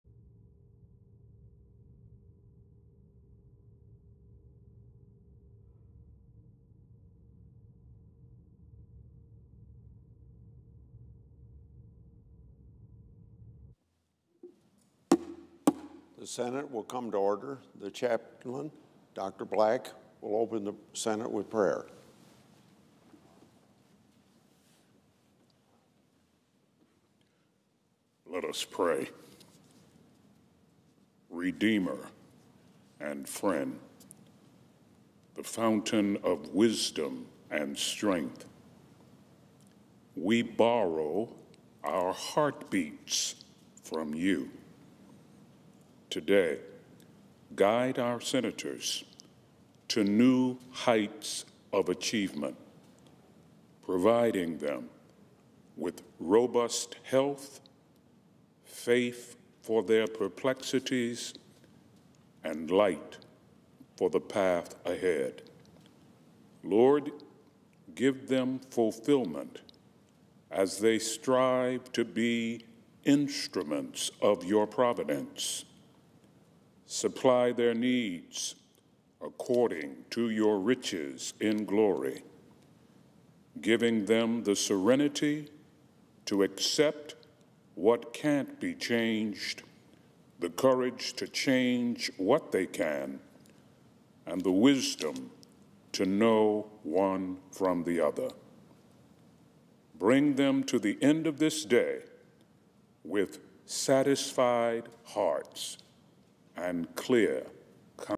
Daily Congress Prayer Senate May 16, 2019
The Chaplain, Dr. Barry C. Black, offered the following prayer:
  Let us pray. Redeemer and friend, the fountain of wisdom and strength, we borrow our heartbeats from You. Today, guide our Senators to new heights of achievement, providing them with robust health, faith for their perplexities, and light for the path ahead.Lord, give them fulfillment as they strive to be instruments of Your providence. Supply their needs according to Your riches in glory, giving them the serenity to accept what can't be changed, the courage to change what they can, and the wisdom to know one from the other. Bring them to the end of this day with satisfied hearts and clear consciences.We pray in Your strong Name. Amen.